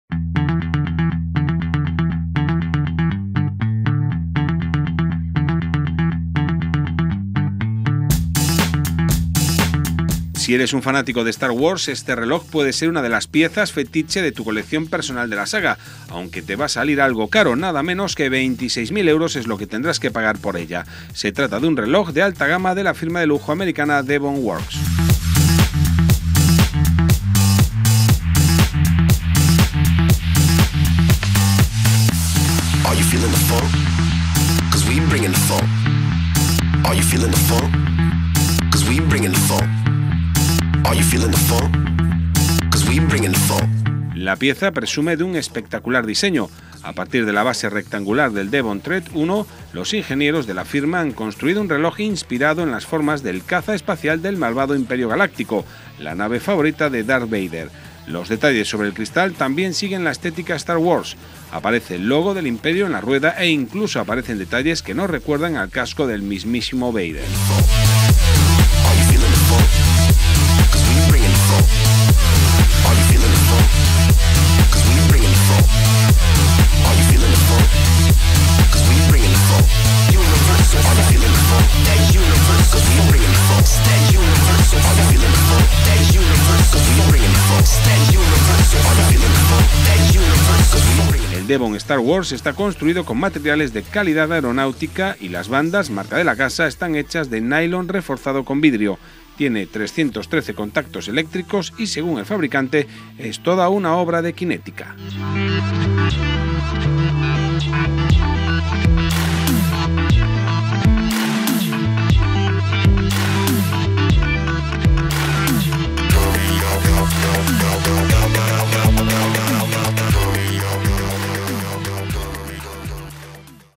LUJO Y TECNOLOGÍA-DEVON RELOJ STAR WARS | By CdRas
Si eres un fanático de Star Wars este reloj puede ser una de las piezas fetiche de tu colección personal de la saga, aunque te va a salir algo caro, nada menos que 26 mil euros es lo que tendrás que pagar por ella. Se trata de un reloj de alta gama de la firma de lujo americana Devon Works.

La pieza presume de un espectacular diseño. A partir de la base rectangular del Devon Tread 1, los ingenieros de la firma han construido un reloj inspirado en las formas del caza espacial del malvado Imperio Galáctico, la nave favorita de Darth Vader. Los detalles sobre el cristal también siguen la estética Star Wars. Aparece el logo del imperio en la rueda e incluso aparecen detalles que nos recuerdan al casco del mismísimo Vader.

El Devon Star Wars está construido con materiales de calidad aeronáutica y las bandas, marca de la casa, están hechas de nailon reforzado con vidrio. Tiene 313 contactos eléctricos y según el fabricante, es toda una obra de arte kinética.